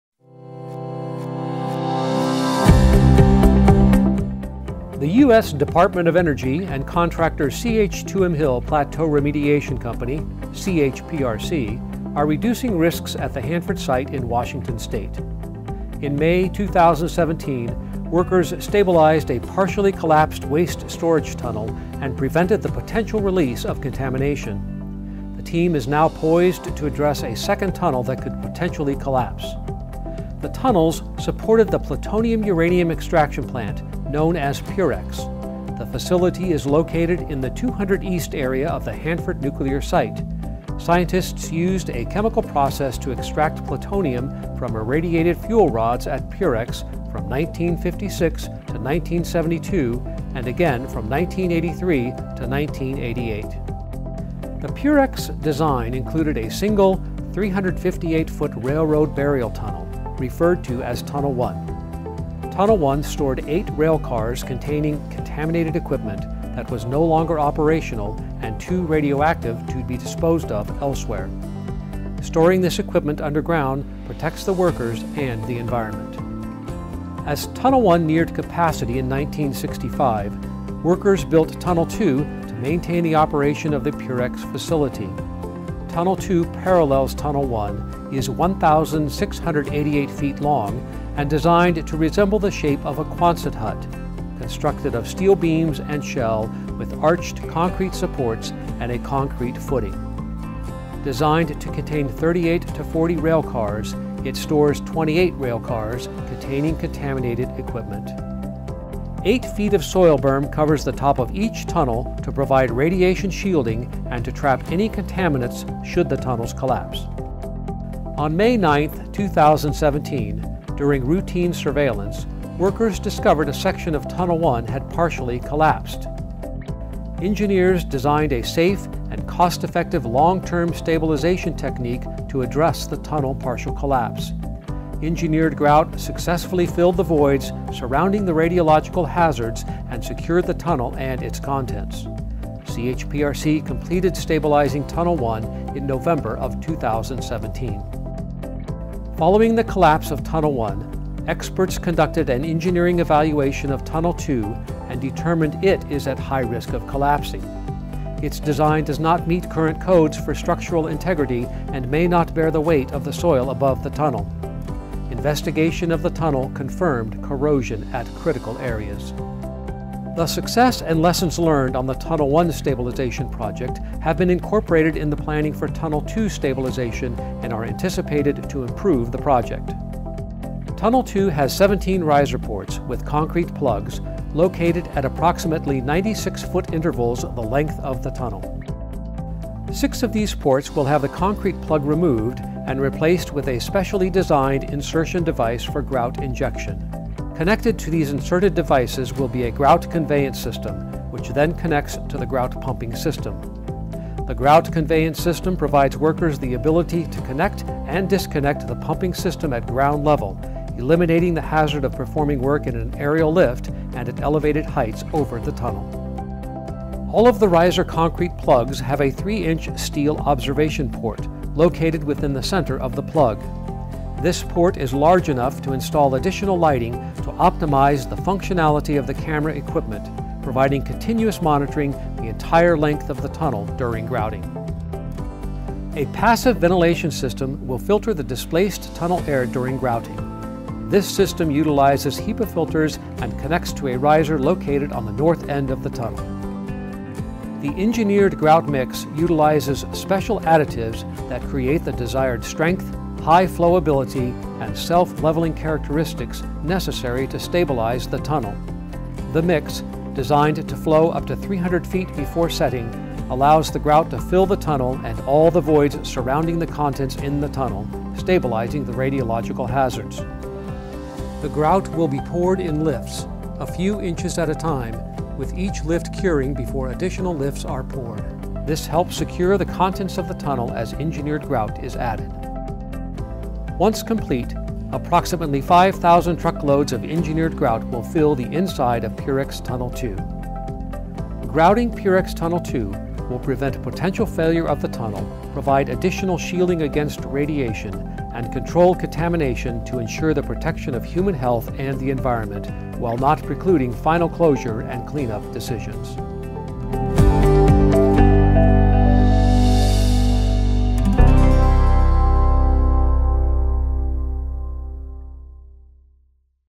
PUREX Tunnel 2 Stabilization Project Animation
This video uses animation to show how DOE and contractor CH2M Hill Plateau Remediation Company would fill waste storage Tunnel 2 near PUREX with grout to prevent a collapse, while allowing for future removal of the waste from the tunnel. For more information on the two waste storage tunnels near the PUREX facility and work being done to stabilize the tunnels, go